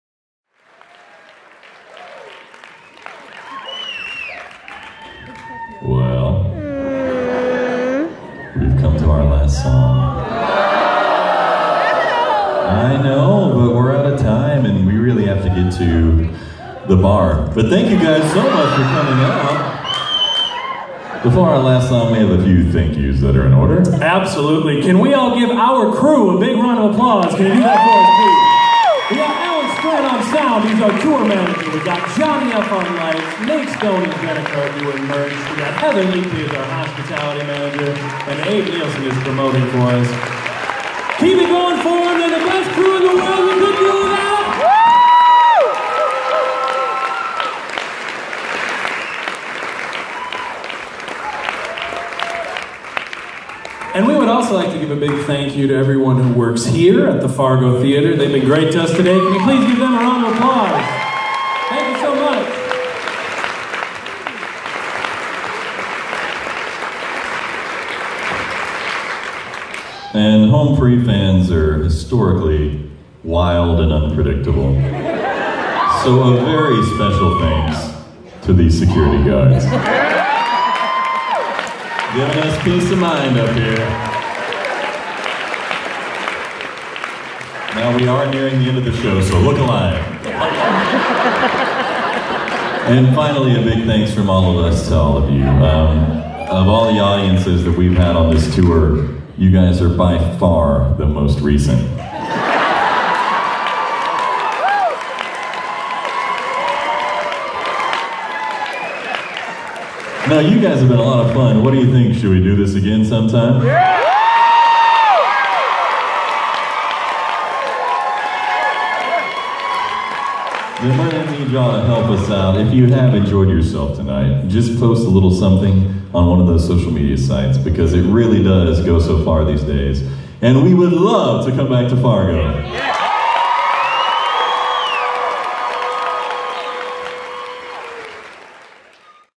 Home Free Thank yous in Fargo, ND 11-8-2015
Home Free singing at the Fargo Theater in Fargo, ND on November 8th, 2015

Their Twitter handles...
Home Free - @HomeFreeGuys
Austin Brown - @_theAustinBrown
Chris Rupp - @HomeFreeHatGuy
Adam Rupp - @AdamHfbeatbox
Tim Foust - @TimFoustMusic
Rob Lundquist - @RobLundquist